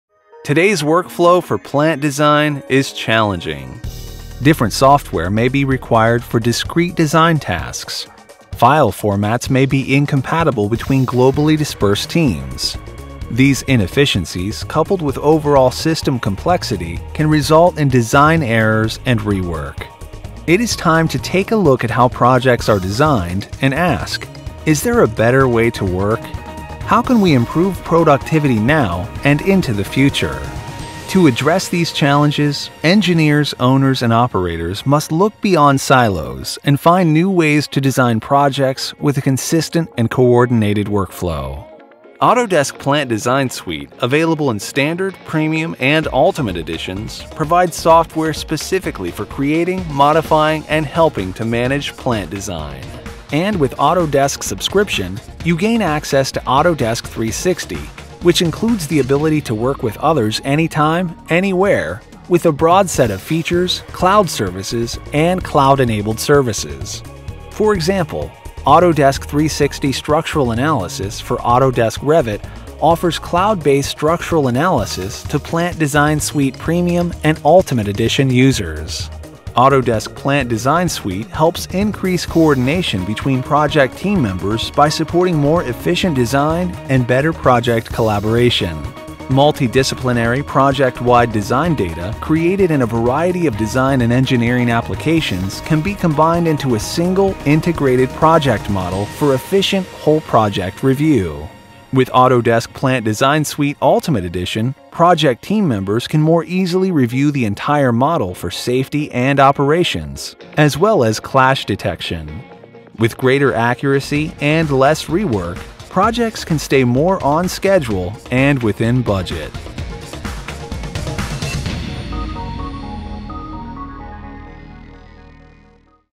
Autodesk Plant Design Suite 2014 overview
Autodesk Plant Design Suite 2014 biedt uitgebreide software voor fabrieksontwerp, modellering en herziening in één gebruiksvriendelijk en betaalbaar pakket. Hiermee kunnen klanten efficiënter ontwerpen en binnen budget en planning blijven. Voortbouwend op de kracht en vertrouwdheid van AutoCAD 2014, biedt de software fabriekspecifieke content en functies voor een hogere productiviteit en betere projectcoördinatie.